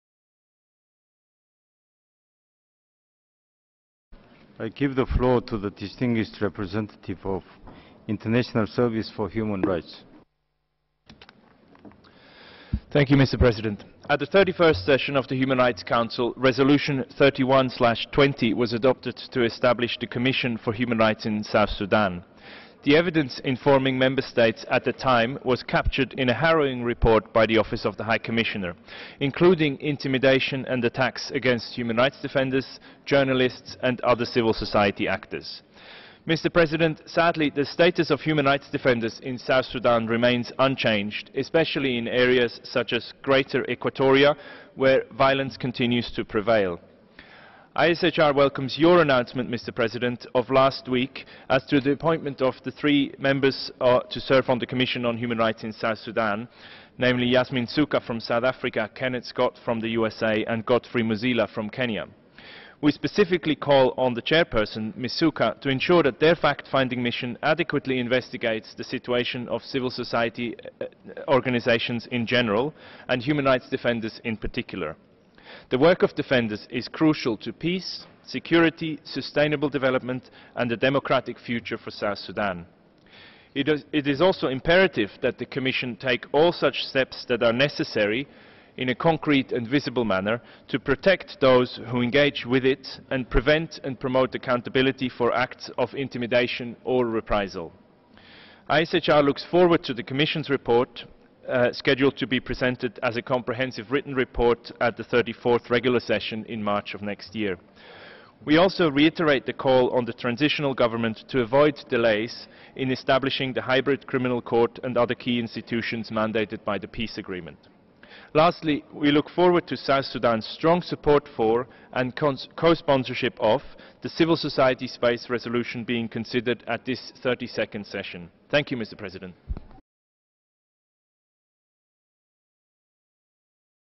ISHR statement on the human rights situation in South Sudan, 22 June 2016
This video is an ISHR statement on the human rights situation in South Sudan delivered on 22 June 2016, at the 32nd Human Rights Council session.